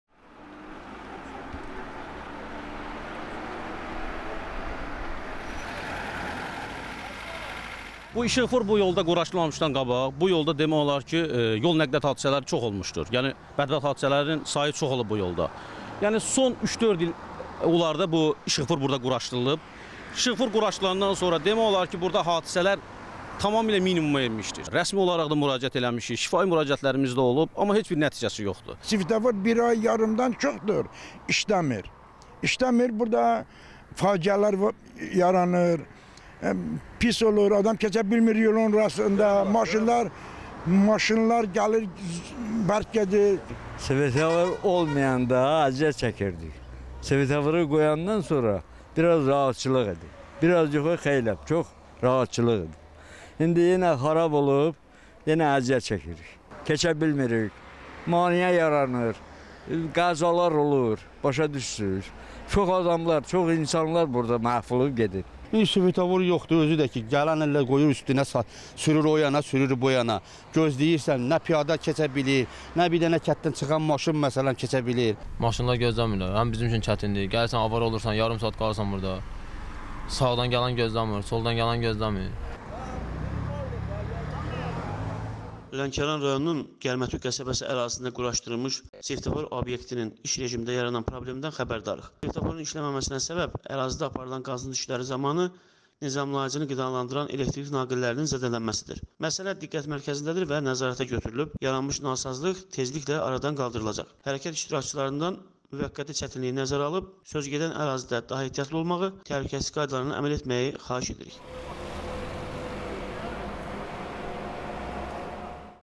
İşıqfor problemi faciə yaratdı | Lənkəran sakinlərinin şikayətinə baxılmır?
Lənkəran rayonunun Gərmətük qəsəbəsi ərazisində quraşdırılan işıqforun işləməməsi sürücülərə, eləcə də piyadalara təhlükə yaradır.
Sakinlərin sözlərinə görə, işıqfor bir aydan çoxdur ki, işləmir və onların aidiyyəti qurumlara müraciətləri nəticəsiz qalıb.
“İşıqfor olmayanda əziyyət çəkirdik. Quraşdırılandan sonra isə rahat hərəkət edə bilirdik. İndi yenə əziyyət çəkirik, yolu keçə bilmirik, qəzalar baş verir. İşıqforun işləməməsi o qədər insanı ölümə aparıb ki...”, – deyə sakin söyləyib.